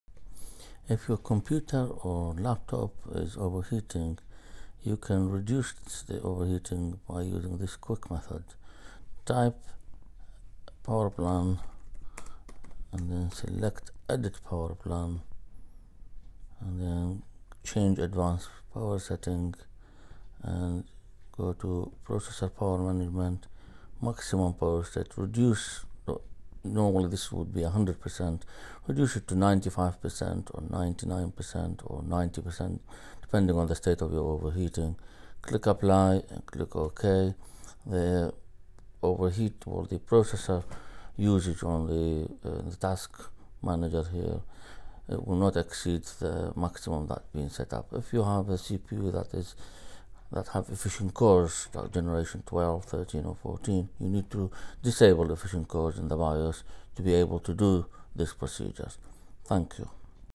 CPU Overheating Control - Quick Fix for Windows 11/10 PC/Laptop Intel/AMD
This tutorial will help you understand how to adjust CPU performance percentage for you pc/laptop in order to adjust the amount of heat released. By reducing the CPU maximum performance from 100% to 99% or lower it will prevent cpu throttling and reduce overheating. This is a quick solution for windows 11/10 that does not require hardware modification and can prove effective in many cases. It suitable for both AMD and intel processors. Please note that if you have intel generation 12th or newer you will need to disable the efficient cores in the bios to be able to reduce the cpu performance as instructed earlier. Please see our other video on new intel processors with e-core technology. For more help and advice please visit our